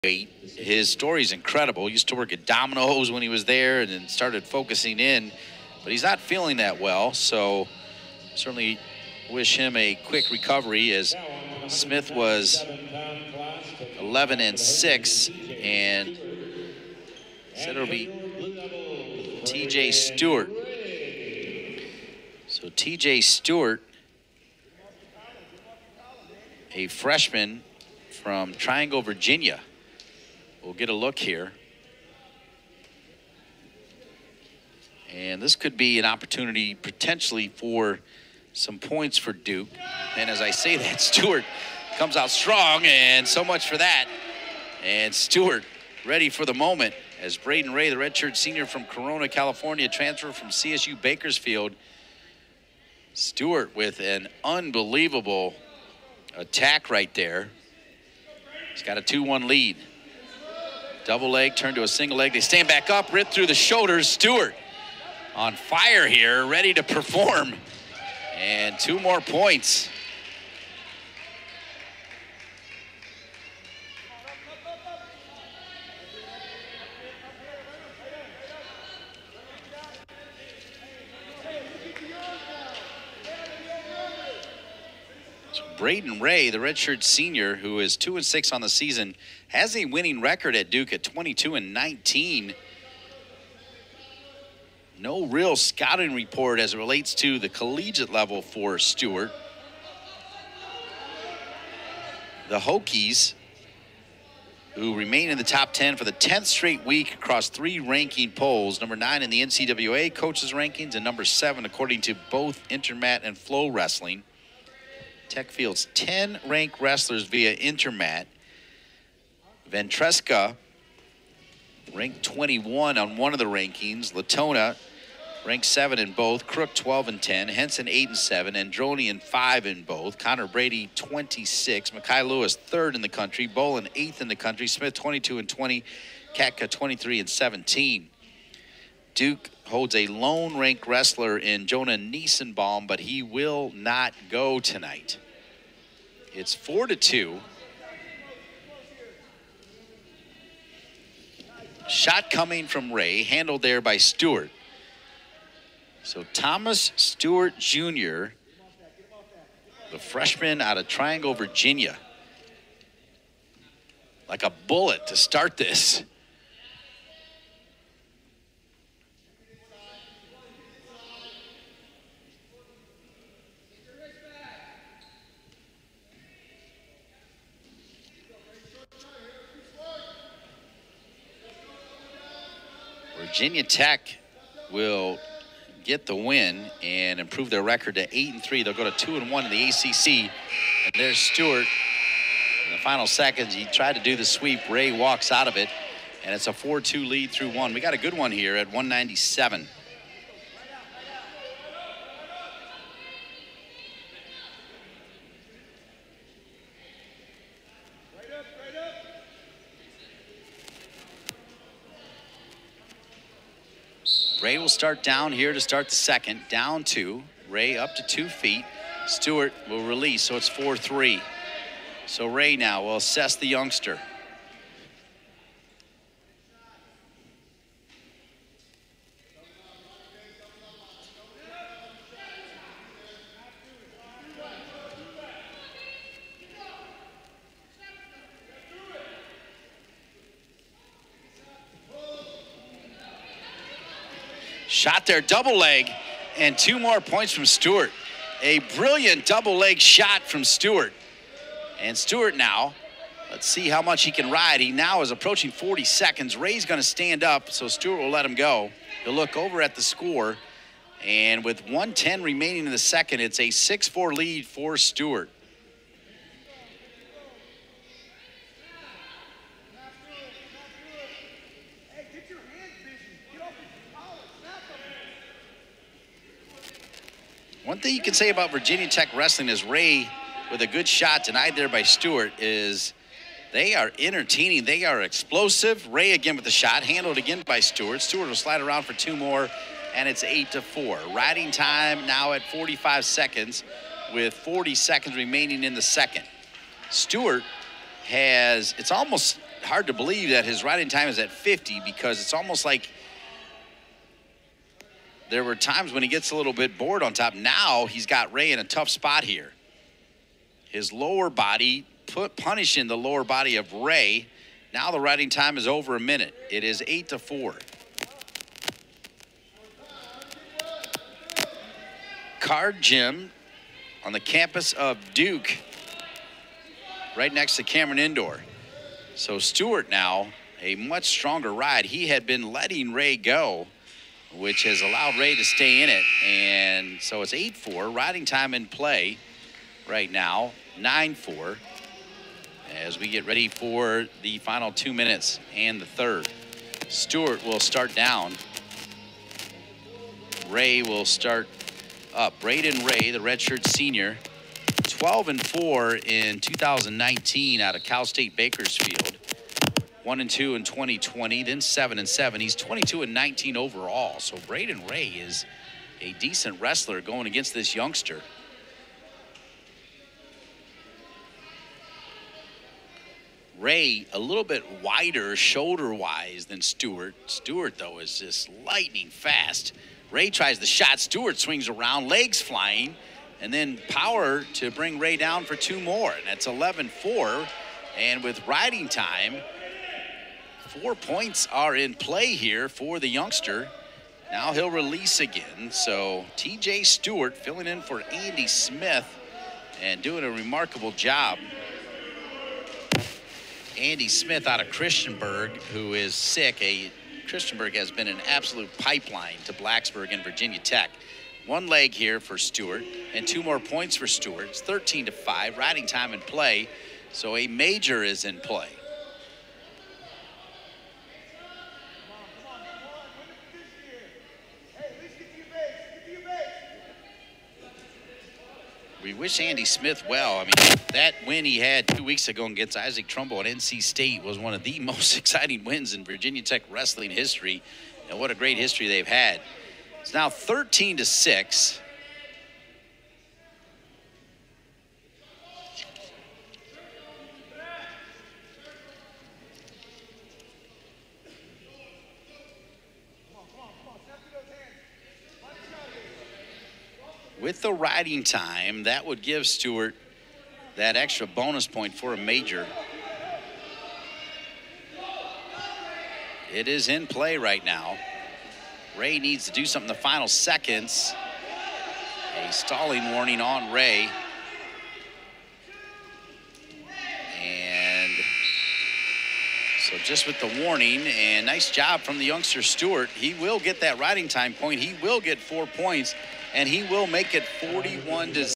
197lbs TJ Stewart (Virginia Tech) vs Brayden Ray (Duke)
2023 Duke vs Virginia Tech wrestling dual
197lbs TJ Stewart (VT) maj dec Brayden Ray (DUKE)